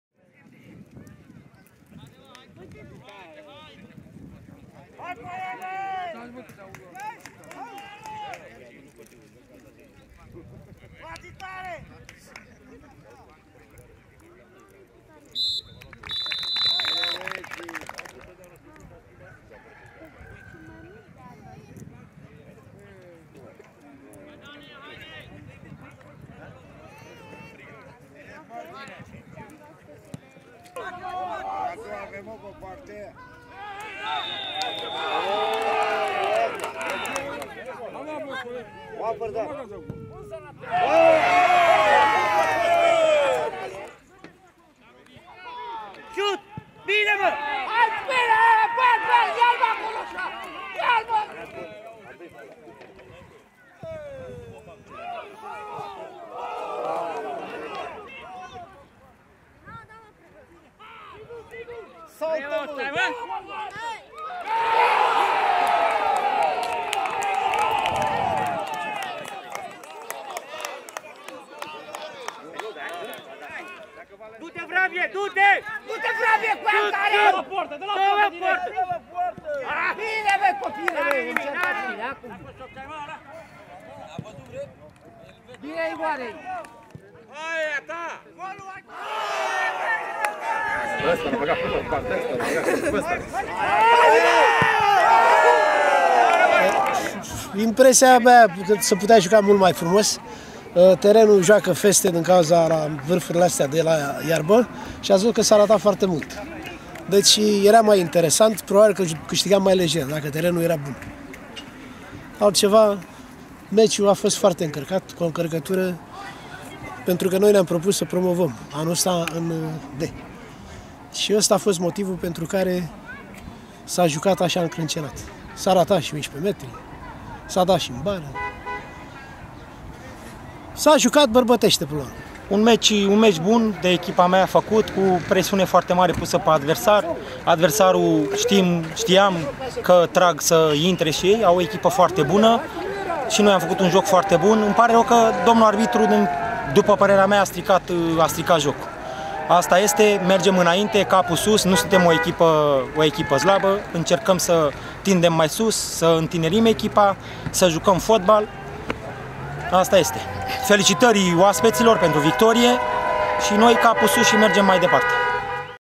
Liga 5 Vest - Olimpia Picior de Munte vs. Săgeata Braniștea 0 - 1
Rezumatul jocului și interviuri